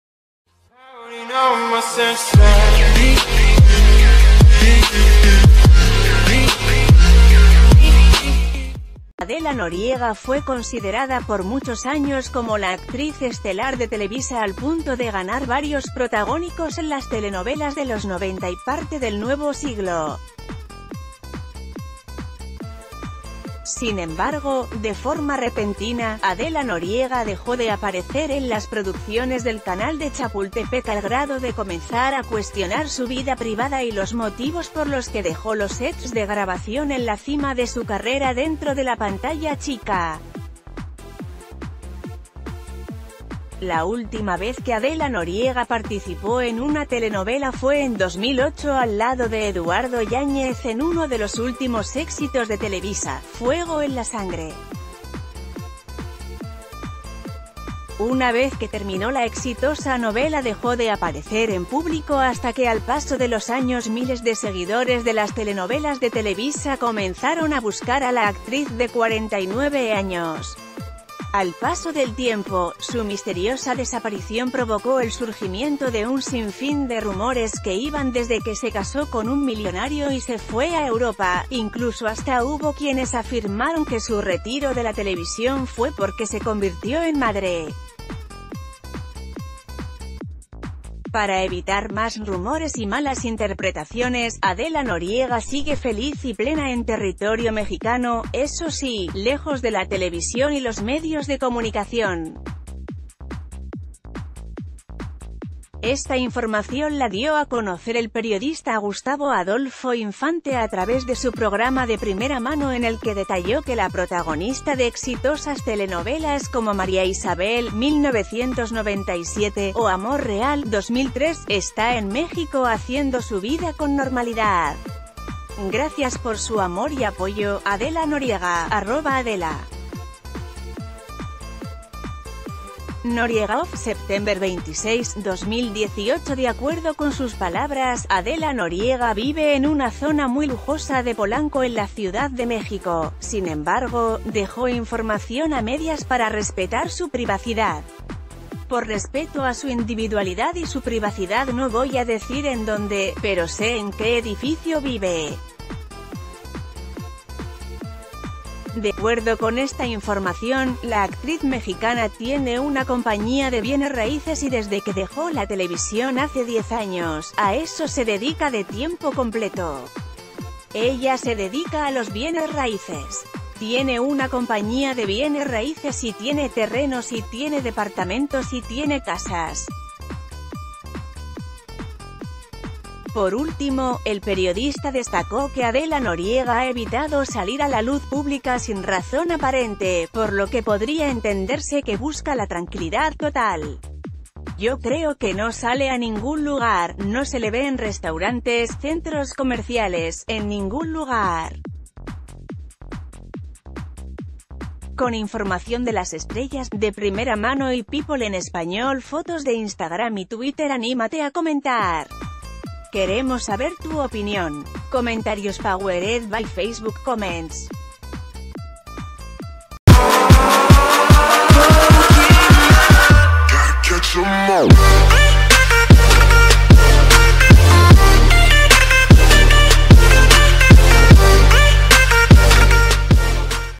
Revelan paradero y ocupación de Adela Noriega tras ausencia en televisión
Adela Noriega fue considerada por muchos años como la actriz estelar de Televisa al punto de ganar varios protagónicos en las telenovelas de los noventa y parte del nuevo siglo. Sin embargo, de forma repentina, Adela Noriega dejó de aparecer en las producciones del canal de Chapultepec al grado de c...